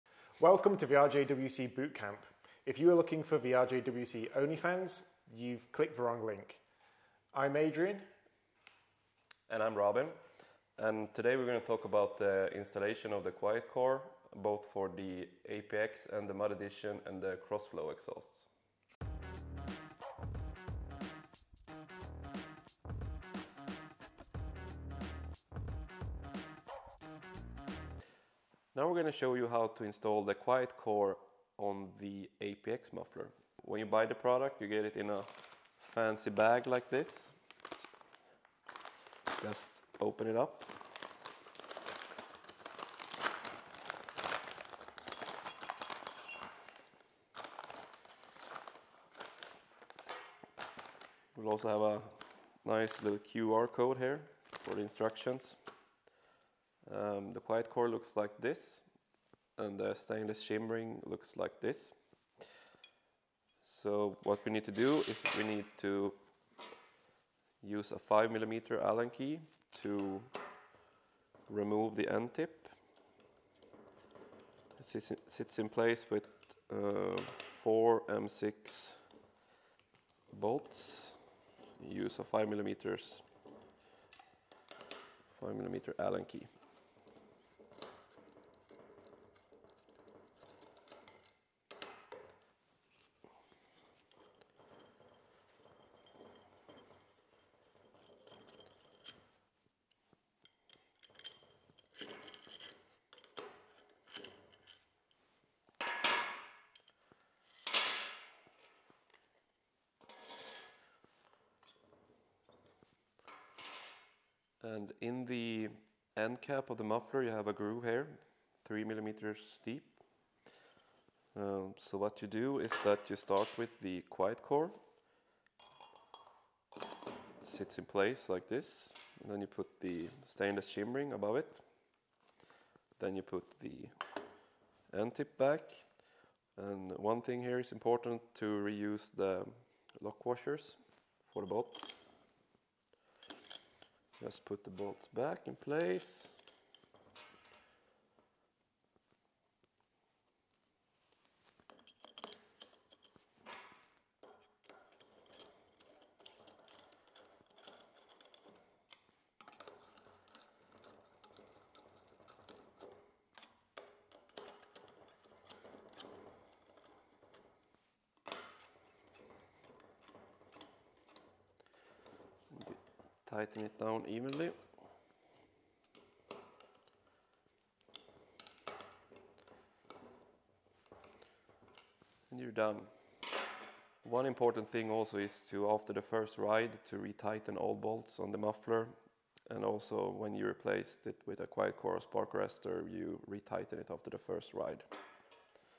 RJWC APX Quiet Core installtion tutorial
Welcome to RJWC Bootcamp! This is the installation procedure for our Quiet Core for our APX line of exhausts. SKU 1051APX.